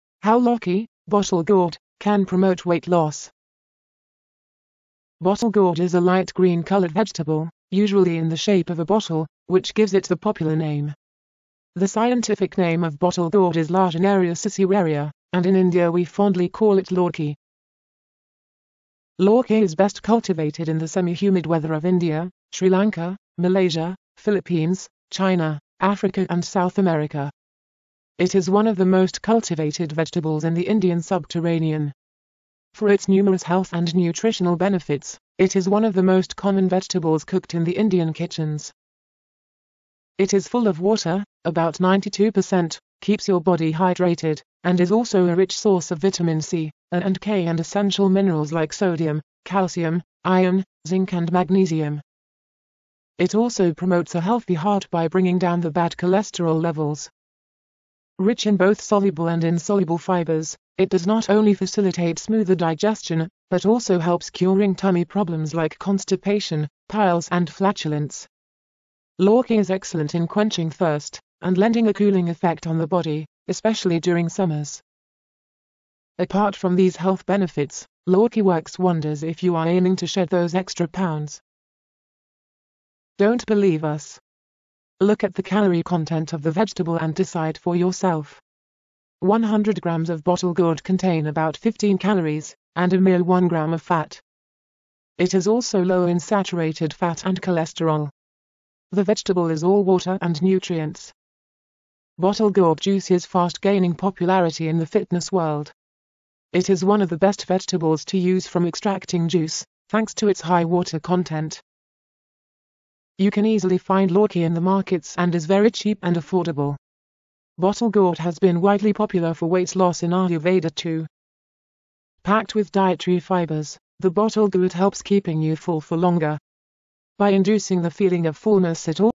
How Lauki Can Promote Weight Loss
Bottle gourd is a light green coloured vegetable, usually in the shape of a bottle, which gives it the popular name. The scientific name of bottle gourd is Lagenaria siceraria,
and in India we fondly call it lauki. Lauki is best cultivated in the semi-humid weather of India, Sri Lanka, Malaysia, Philippines, China, Africa and South America. It is one of the most cultivated vegetables in the Indian subterranean. For its numerous health and nutritional benefits, it is one of the most common vegetables cooked in the Indian kitchens.
 It is full of water (about 92%), keeps your body hydrated, and is also a rich source of vitamin C, A and K and essential minerals like sodium, calcium, iron, zinc and magnesium. It also promotes a healthy heart by bringing down the bad cholesterol levels. Rich in both soluble and insoluble fibers, it does not only facilitate smoother digestion, but also helps curing tummy problems like constipation, piles and flatulence. Lauki is excellent in quenching thirst, and lending a cooling effect on the body, especially during summers.
Apart from these health benefits, Lauki works wonders if you are aiming to shed those extra pounds. Don’t believe us? Look at the calorie content of the vegetable and decide for yourself. 100 grams of bottle gourd contain about 15 calories, and a mere 1 gram of fat. It is also low in saturated fat and cholesterol.

Lauki juice is the perfect low-fat aid for a multitude of health benefits. For best results you should have the juice in the morning on an empty stomach. Also, one should consume the juice immediately as it oxidises quite fast. You should not strain the juice with a sieve as you will then lose out on the fiber, which is the key to shed extra kilos.
Asha Thorat in her book 25 Fat Burning Juice Recipes
writes, “Fiber is the key to weight loss. Bottle gourd contains less calories with no fat. It also contains essential vitamins and minerals like Vitamin C, Vitamin B, Vitamin K, Vitamin A, Vitamin E, Iron, Folate, Potassium and Manganese.”

Bottle gourd is also known to help you sleep better, which is very crucial for weight loss. Having good sleep activates your hormones and improves your metabolism.
With the hot summer months are knocking at the doorstep, you know which vegetable to stock your kitchen with. Beat both the heat and weight with this wonder vegetable.